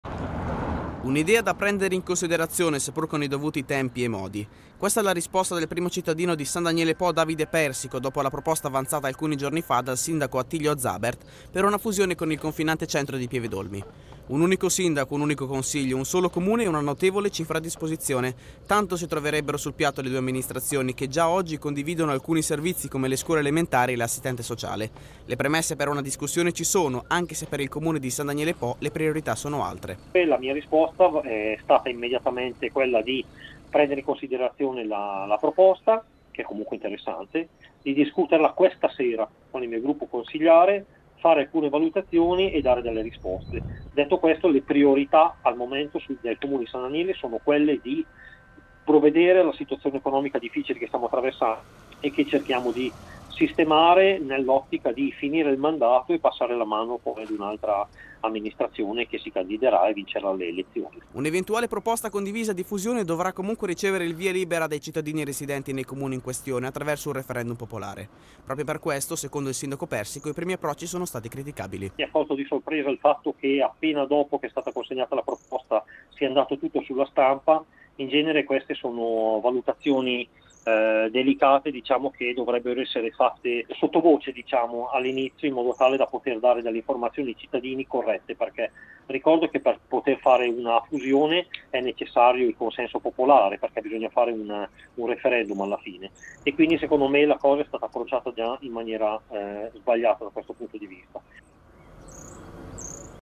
Ipotesi fusione San Daniele - Pieve D'Olmi. Persico prende tempo
Dopo la proposta di fusione tra i comuni di Pieve d'Olmi e San Daniele Po, il sindaco Davide Persico non si sbilancia."Ad oggi non è tra le nostre priorità, ma ne parlerò questa sera con il mio gruppo consigliare" ha detto il primo cittadino di San Daniele